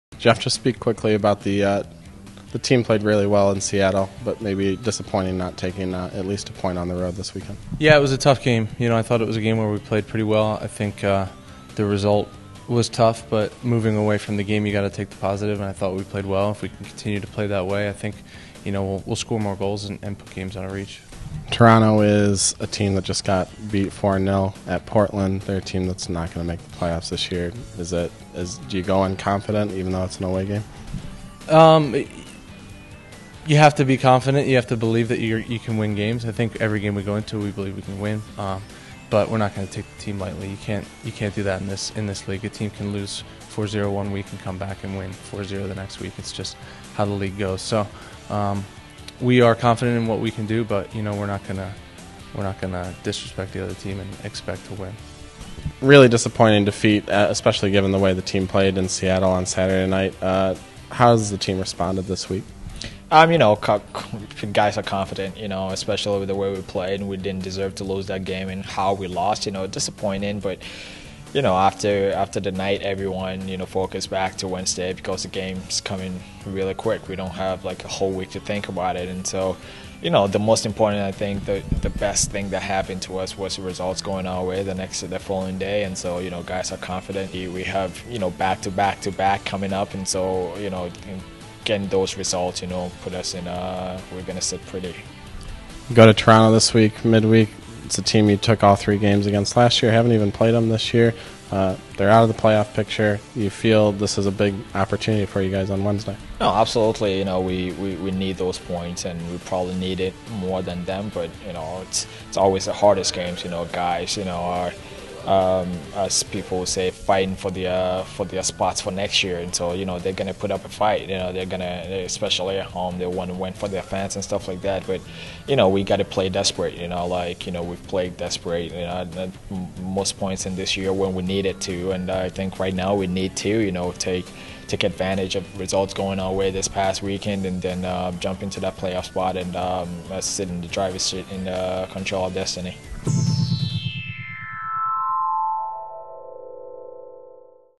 Match Preview | Larentowicz and Nyarko discuss Seattle and Toronto
Chicago Fire midfielders Jeff Larentowicz and Patrick Nyarko talk about the loss at Seattle and the upcoming match against Toronto.